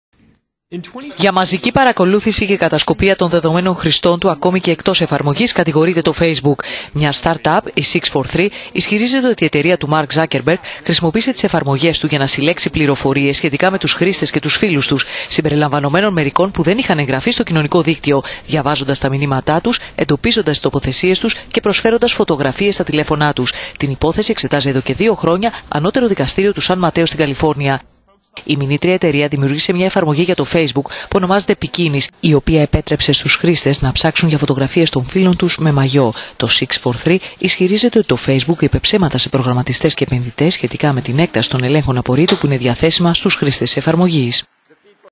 Για «κατασκοπεία» των χρηστών του κατηγορείται το Facebook
Μια start up εταιρία μηνύει την εταιρία του Μαρκ Ζάκερμπεργκ ότι παρακολουθούσε μαζικά τα δεδομένα των χρηστών του ακόμη και εκτός εφαρμογής…